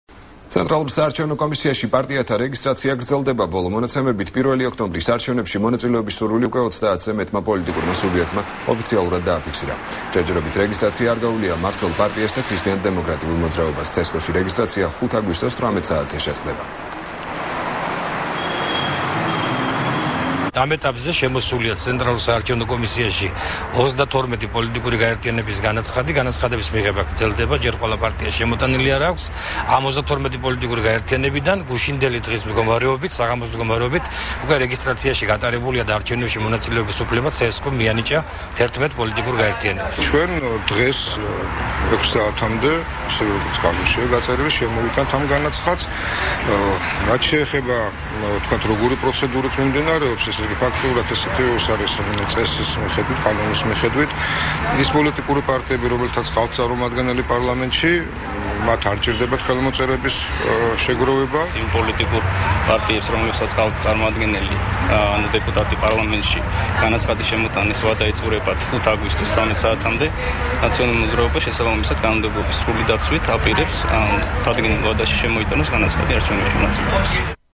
ახალი 3 | საარჩევნო განაცხადები | 03.08.12
ცენტრალურ საარჩევო კომისიაში პარტიათა რეგისტრაცია გრძელდება. ბოლო მონაცემებით 1 ოქტომბრის არჩევნებში მონაწილეობის სურვილი უკვე 30-ზე მეტმა პოლიტიკურმა სუბიექტმა ოფიციალურად დააფიქსირა. ჯერჯერობით რეგისტრაცია არ გაუვლია მმართველ პარტიას და ქრისტიან დემოკრატიულ მოძრაობას. ცესკოში რეგისტრაცია 5 აგვისტოს 18 საათზე შეწყდება.